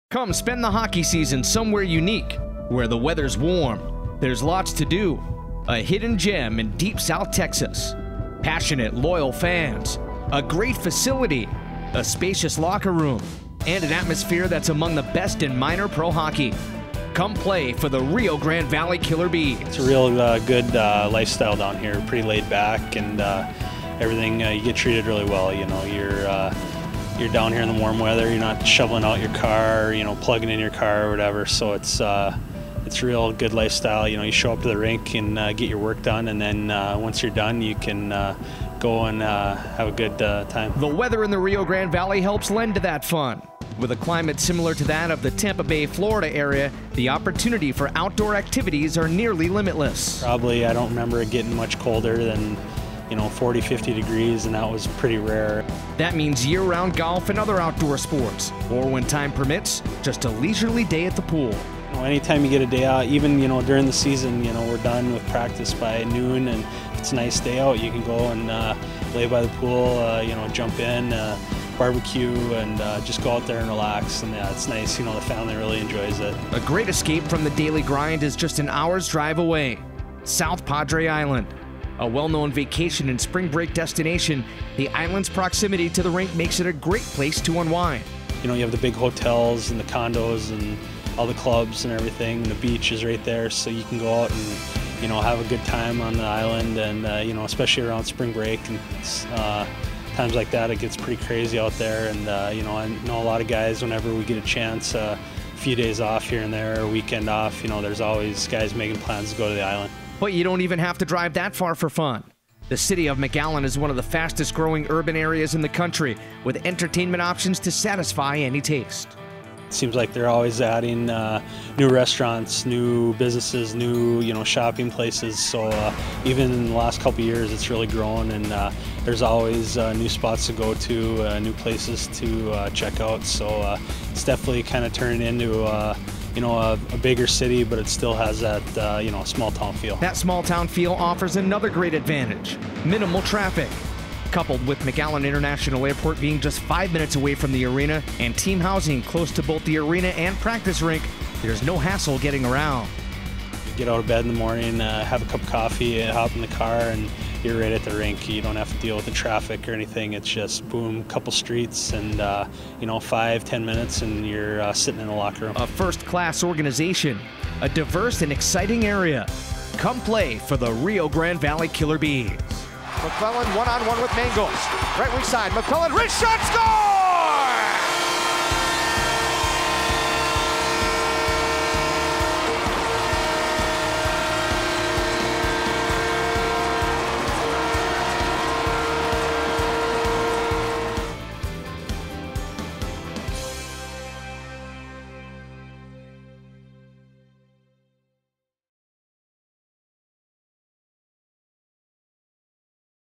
Play for the Killer Bees; A look at the RGV
Come play for the Rio Grande Valley Killer Bees in the Central Hockey League. South Texas is a growing area; find out more about it here.